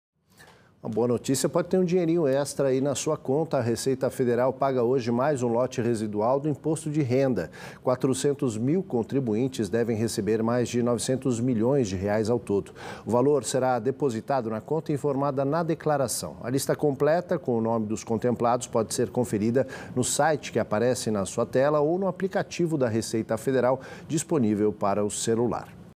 Refeita Federal paga mais um lote residual do Imposto de Renda
Nesta quinta-feira (29), a Receita Federal paga mais um lote residual do Imposto de Renda. 400 mil contribuintes devem receber mais de R$ 900 milhões.